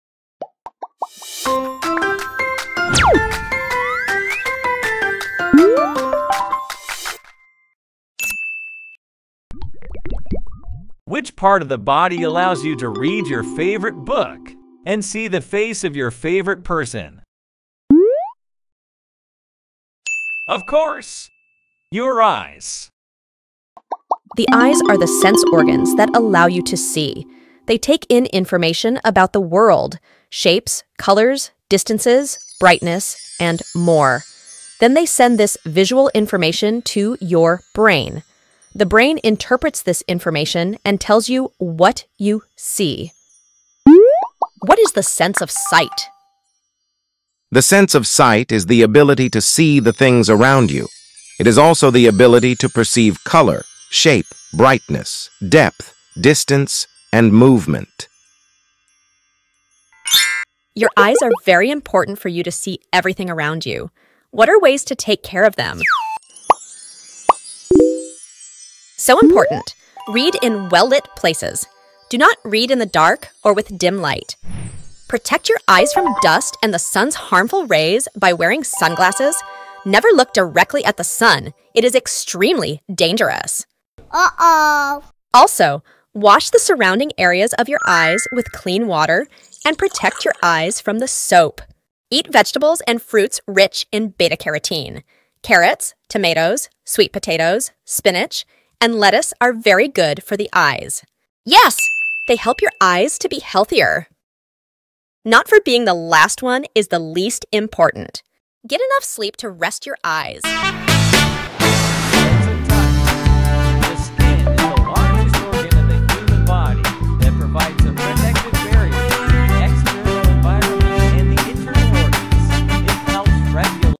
Sense of sight | The eyes 👀 are the sensory organs for the sense of sight
Take a look around you. What do you see? You might see a computer or phone with a shining, colorful screen. A piece of paper may be under your left hand and a sharpened pencil in your right hand. While you look at these objects with your eyes, your brain is what is recognizing the objects. Many people take sight for granted, but how are you able to see and register objects?

You probably already know that your body has five senses that help you experience the world around you. These senses are touch, taste, hearing, smell, and sight. Although all of your senses are important, many people think that sight would be the most difficult one to live without. The eyes are the sensory organs to help you use the sight sense. The eyes capture the information that you see and then they transfer that information to your brain 🧠 throughout nerve singnals and the brain tells you the colors, shapes and distances that you see. It’s very important to protect your eyes 👀.

If you could not see, how would you watch TV, cook food and not burn yourself, or walk across the street without being hit by a car? Many people do all kinds of activities without being able to see. Let's learn a bit more about how vision works.

Audios VOICES with ElebenLabs